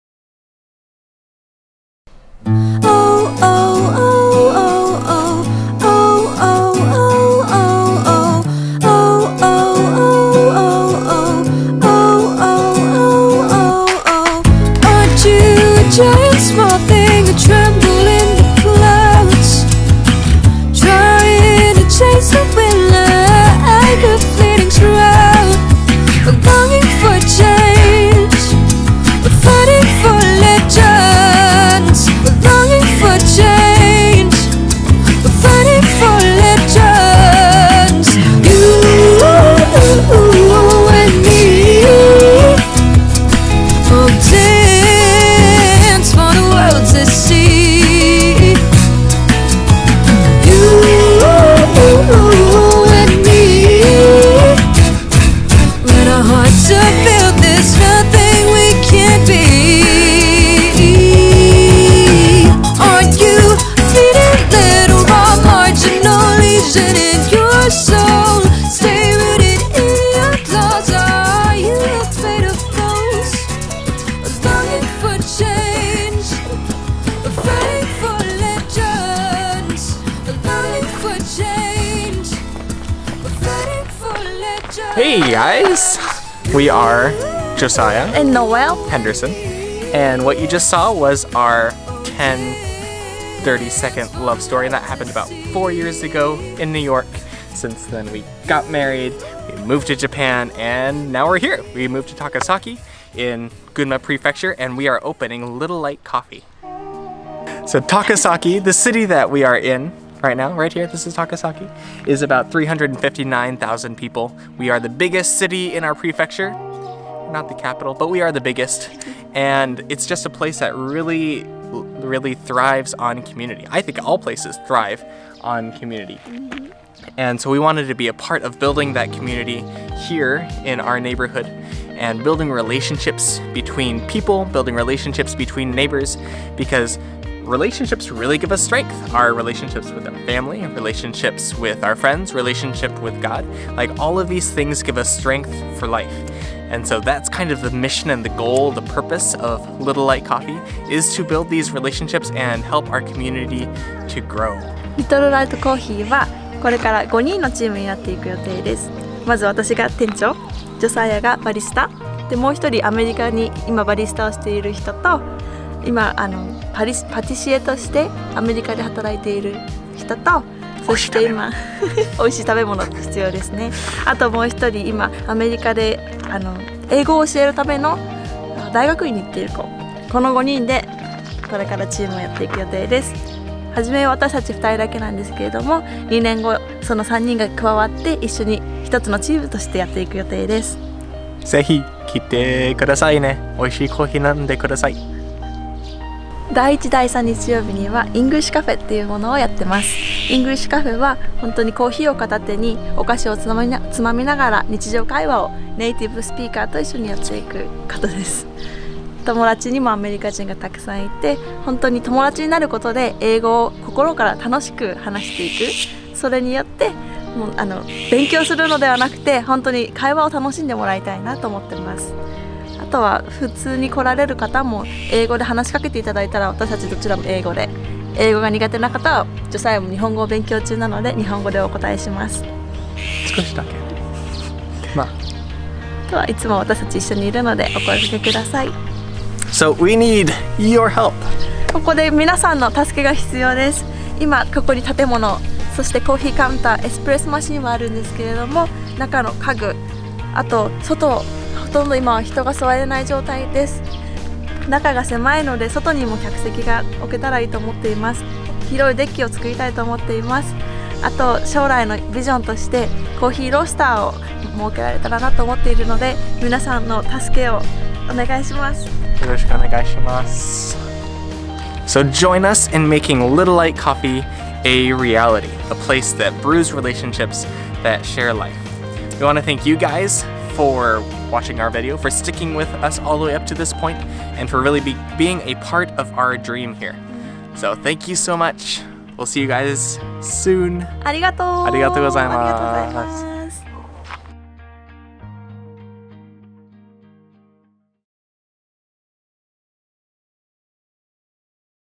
Welcome to Little Light Coffee - Japanese
Little Light Coffee is officially (though not literally) off the ground! Be a part of our team and help us make our vision for Little Light a reality!